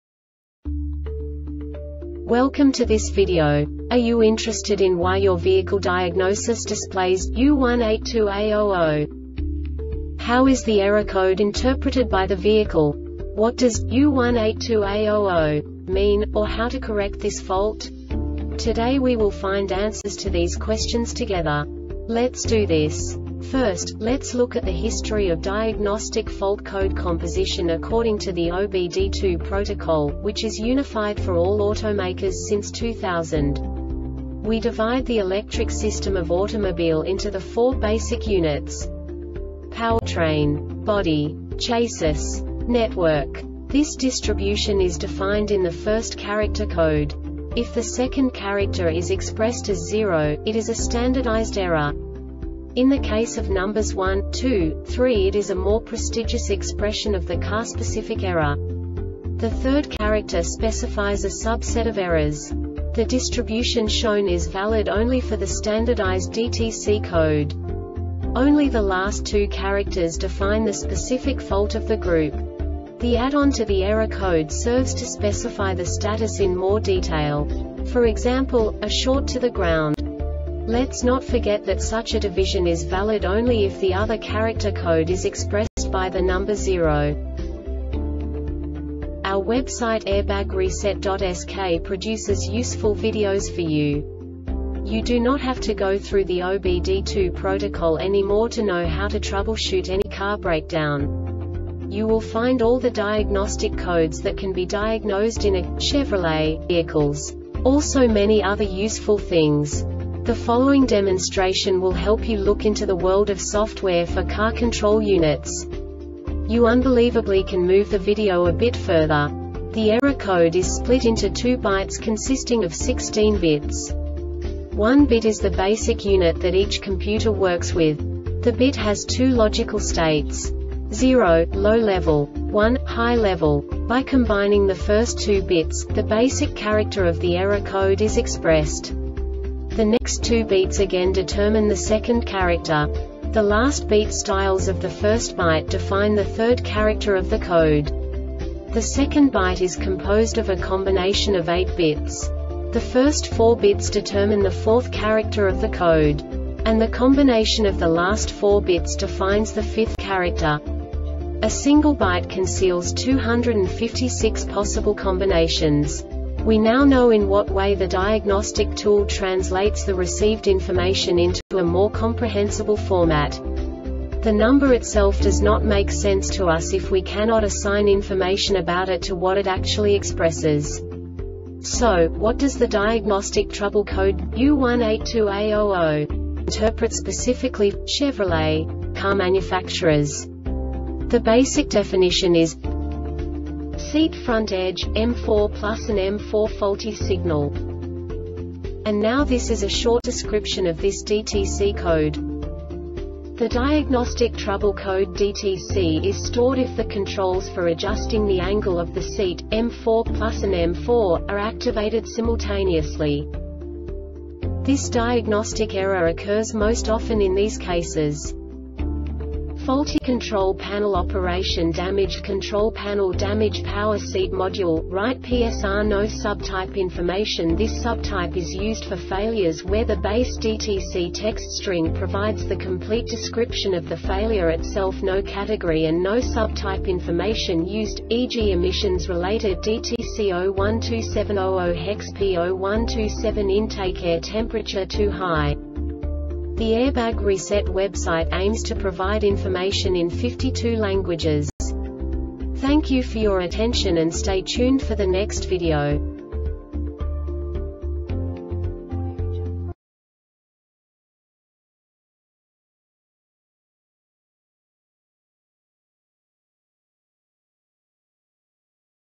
DTC Chevrolet U182A-00 Short Explanation
Contents:
0:21 Basic DTC analysis according to OBD2 protocol standard.
1:48 Insight into programming
2:58 A diagnostically understandable description of the Diagnostic Trouble Code
3:05 Basic error definition

Make:
Chevrolet
Code:
U182A 00
Definition:
Seat Front Edge, M4+ and M4-. Faulty Signal
Description:
The diagnostic trouble code (DTC) is stored if the controls for adjusting the angle of the seat,M4+ and M4-,are activated simultaneously.
Cause:
Faulty control panel operation.Damaged control panel.Damaged power seat module,right (PSR).